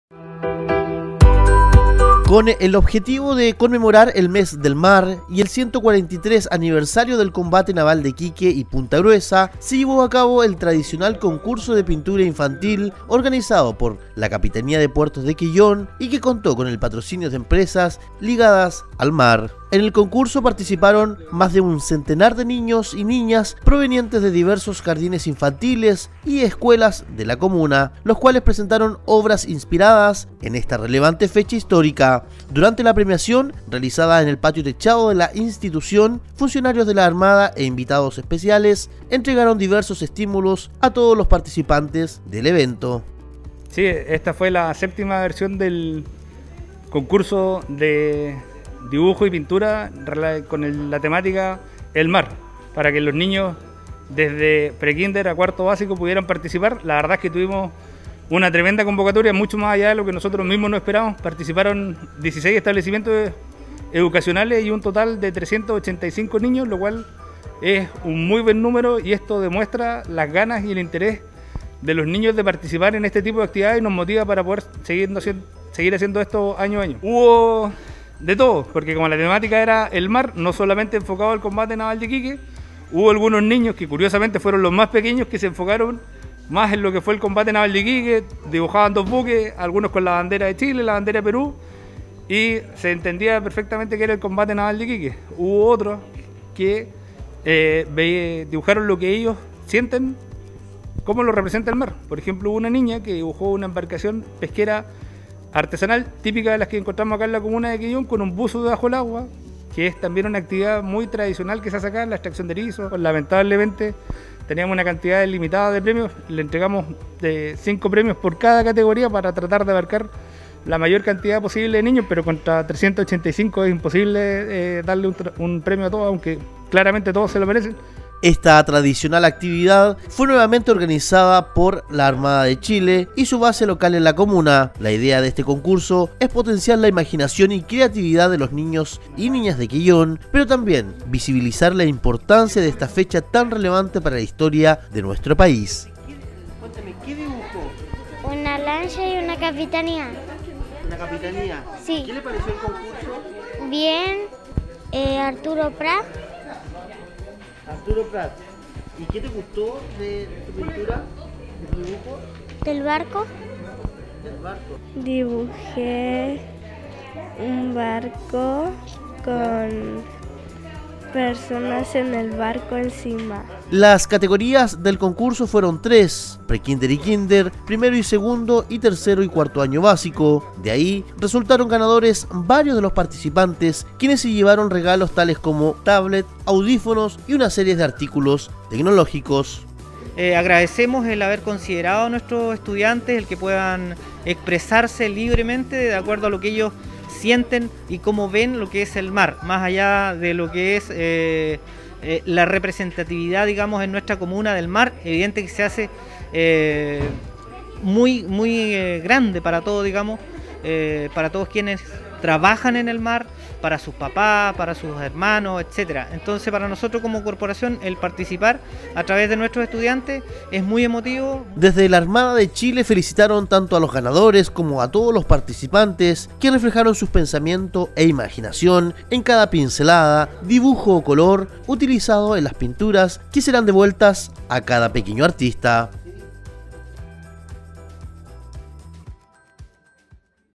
Armada premia a niños y niñas de Quellón en concurso de pintura | Quellón Informa
▶️ Una iniciativa, que se desarrolla en el marco de la conmemoración de los 143 años del combate naval de Iquique, busca que los participantes, estudiantes de diferentes establecimientos de la comuna grafiquen en sus dibujos el entorno marítimo relacionado con la Armada de Chile. Este año participaron un centenar de niños y niñas.